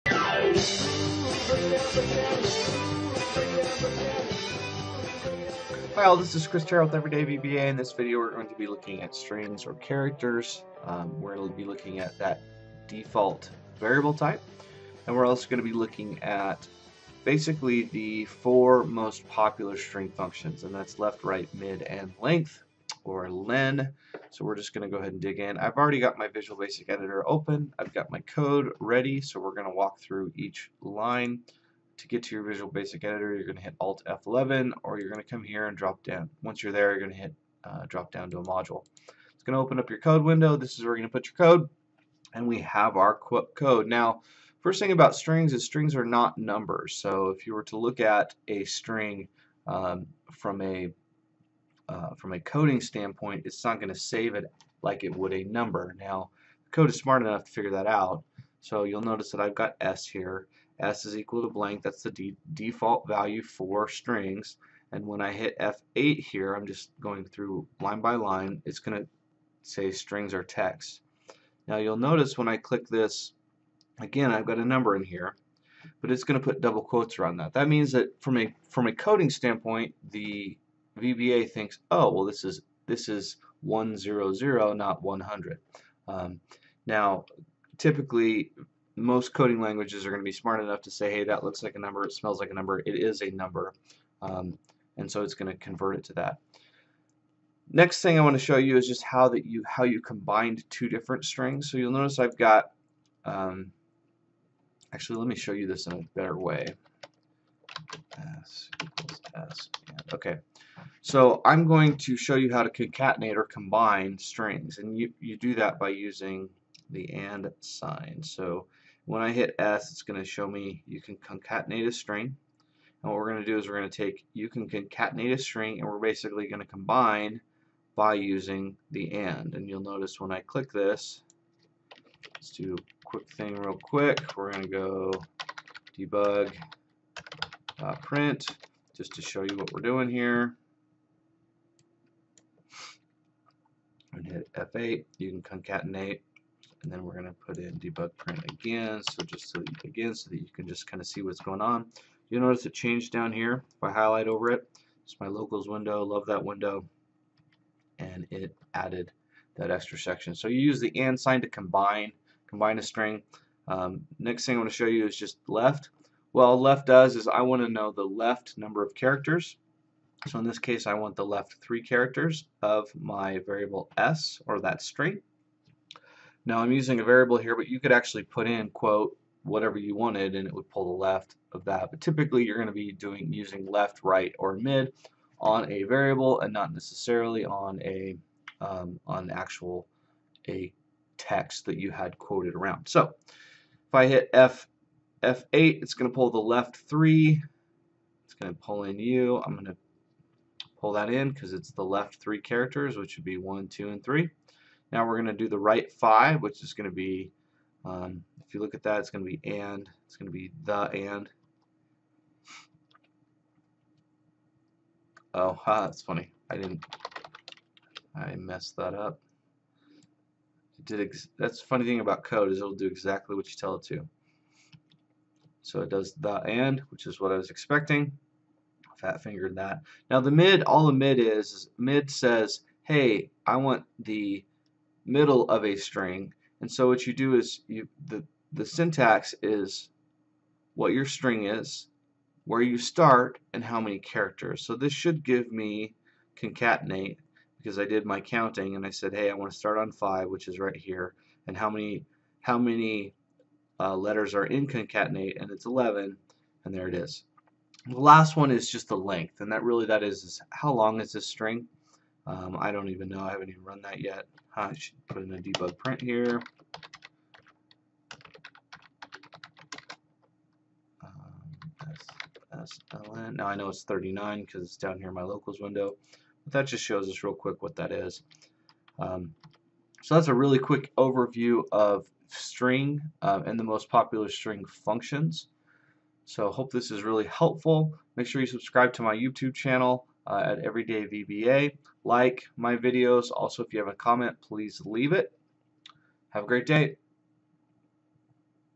Using the Excel VBA String Variable - Concatenating, Left, Right, Mid, and Len Functions
You can do a lot with the string variable type.  In this video we declare the variable type, use the & to concatenate a string and use the Left, Right, Mid, and Len functions.  Master these and you will have mastered Strings in VBA

Code:
Sub Variables()

' STRINGS OR CHARACTERS
Dim s As String
'Default = ""
s = "Strings are for Text"
s = "100"                   'Strings can hold numbers and sometimes are smart
                            'enough to know they are numbers

s = "You concatenate a String "
Debug.Print s
s = s & "by Using the &"
Debug.Print s

'Left the Left number of Characters of a string
sl = Left(s, 3)
Debug.Print sl

'Right the Right Number of Characters of a String
sr = Right(s, 5)
Debug.Print sr

'Mid the Middle Number of Characters of a String from a starting point
sm = Mid(s, 5, 11)
Debug.Print sm

'Len the Length of a string
slen = Len(s)
Debug.Print slen

End Sub